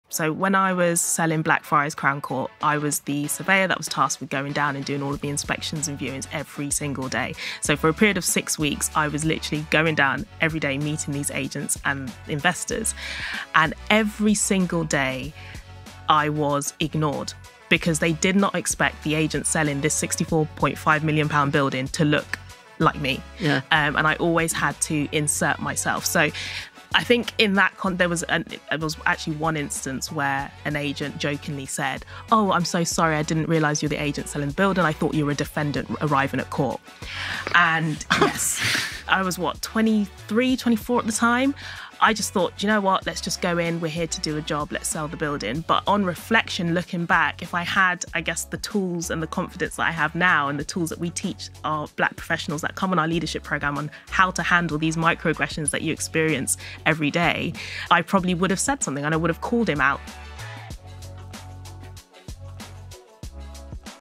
Accelerating Careers in Real Estate podcast DE&I Allyship special | Faith's experience
Accelerating Careers in Real Estate podcast Allyship special episode featuring: Sue Brown Real Estate Balance, Gafar Fasola British Property Futures, Vanessa Murray Circle Partnership and Faith Locken We Rise In

Supported by Macdonald & Company